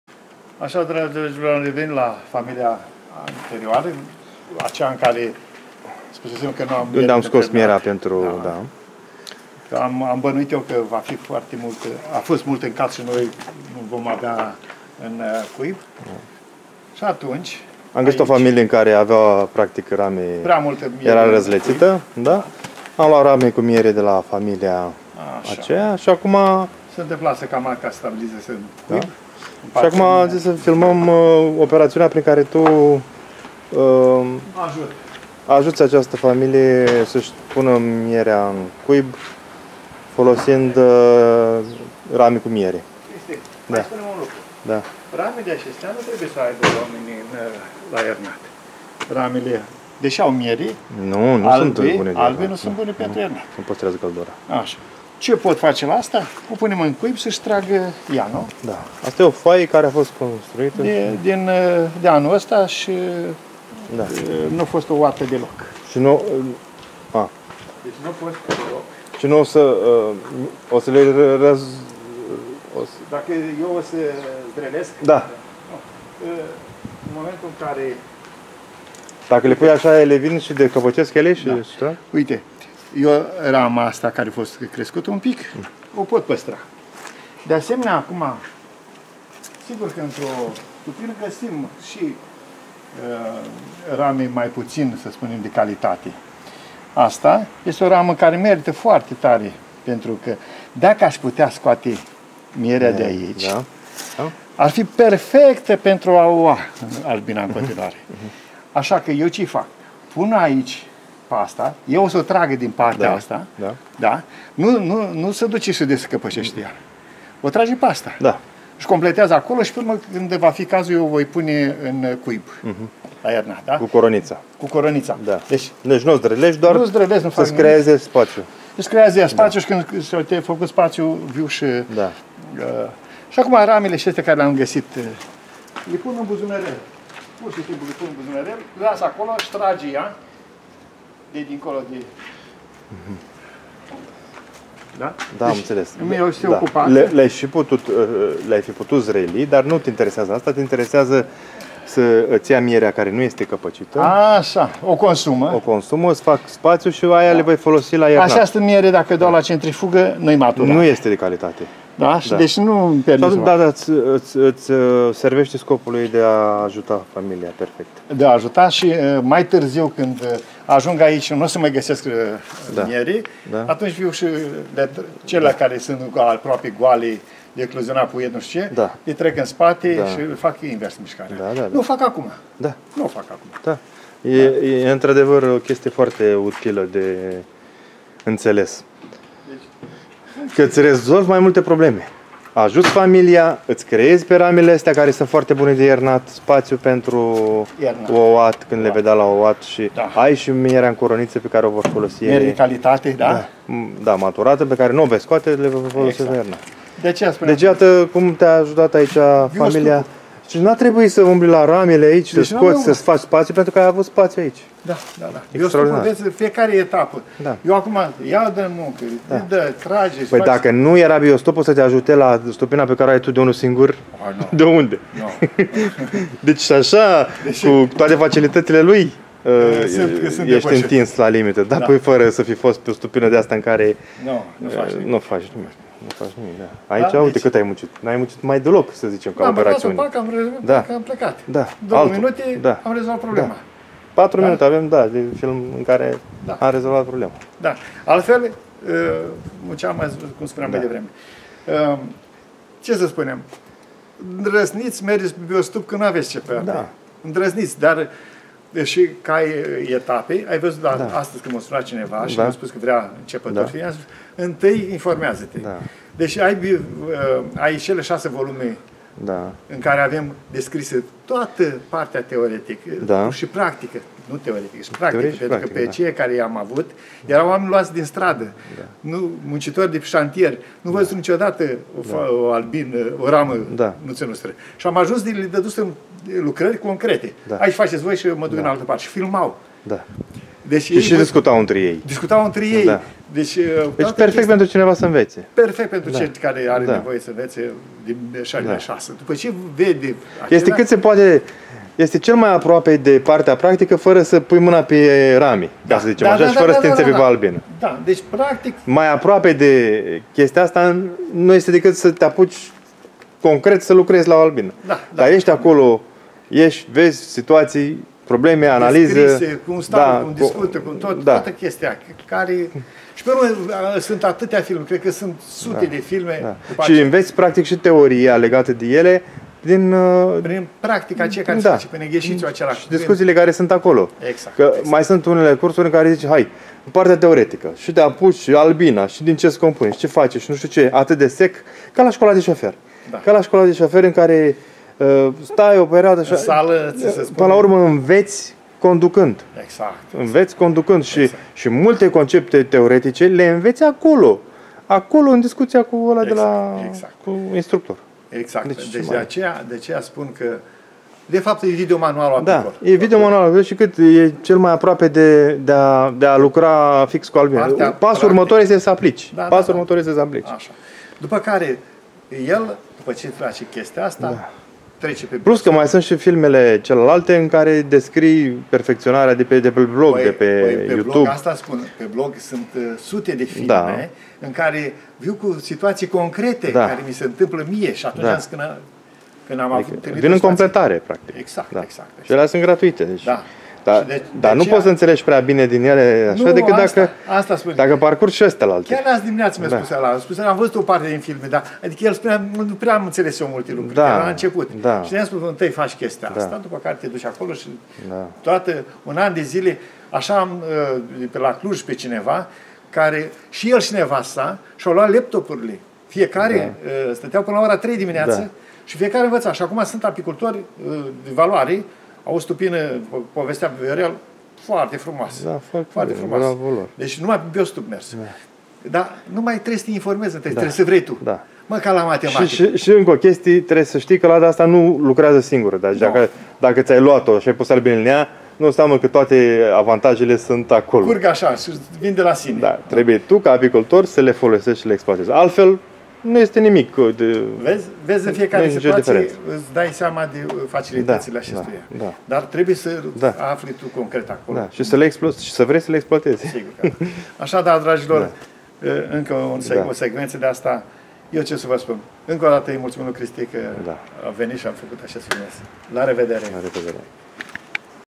P2 10 Aug 2017 Solutia la filmul P1 Hranirea stimulativa Familii de vanzare Selectia Rame
Daca filmele va sunt utile, as aprecia un Like - Suntem pe 10 August 2017 la stupina si va prezint solutia din filmul - P1 10 Aug 2017 Film cu multe informatii: Miere maturata Familii de vanzare Organizarea cuibului Rame. Daca filmul va este util, as aprecia un Share si Like - VIDEO APICULTURA cu Proful Online sau cautati pe Google: profu online Ioan Ursu - Vizitati si